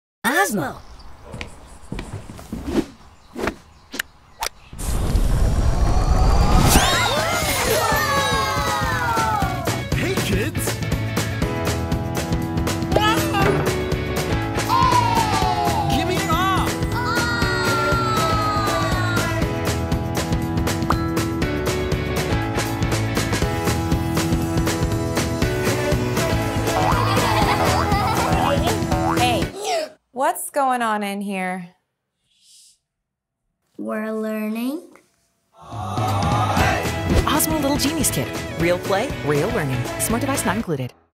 Osmo Little Genius Kit
Här kan den allra minsta lära sig bokstäver, siffror, färg och form i förskolanivå. Ålderanpassat ordföråd och direkt gensvar när det blir rätt. Här får vi både minnas, lyssna och titta på färg och form och lära oss medans vi provar oss fram genom de olika uppgifterna och spelen.